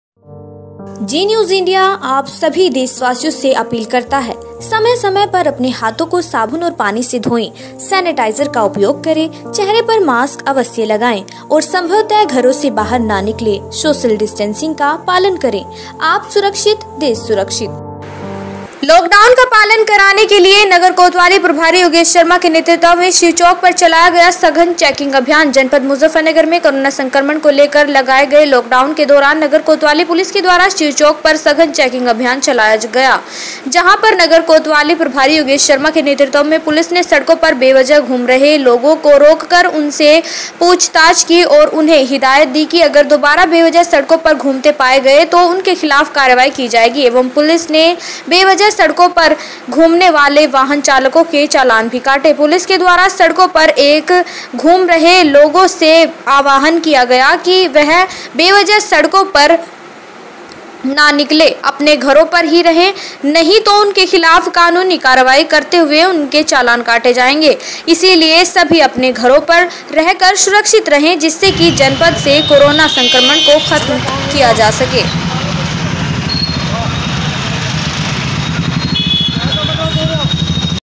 शिव चौक पर चलाया सघन चेकिंग अभियान
लॉक डाउन का पालन कराने के लिए नगर कोतवाली प्रभारी योगेश शर्मा के नेतृत्व में शिव चौक पर चलाया सघन चेकिंग अभियान

जनपद मुजफ्फरनगर में कोरोना संक्रमण को लेकर लगाए गए लोक डाउन के दौरान नगर कोतवाली पुलिस के द्वारा शिव चौक पर सघन चेकिंग अभियान चलाया जहां पर नगर कोतवाली प्रभारी योगेश शर्मा के नेतृत्व में पुलिस ने सड़कों पर बेवजह घूम रहे लोगों को रोककर उनसे पूछताछ की और उन्हें हिदायत दी कि अगर दोबारा बेवजह सड़कों पर घूमते पाए गए तो उनके खिलाफ कार्रवाई की जाएगी एवं पुलिस ने बेवजह सड़कों पर घूमने वाले वाहन चालकों के चालान भी काटे पुलिस के द्वारा सड़कों पर एक घूम रहे लोगों से आह्वान किया कि वह बेवजह सड़कों पर मैं अपने घरों पर रहे नहीं तो उनके खिलाफ कानूनी कार्रवाई करते हुए उनके चालान काटे जाएंगे इसलिए सभी अपने घरों पर रहकर सुरक्षित रहे जिससे कि जनपद से कोरोना संक्रमण खत्म हो सके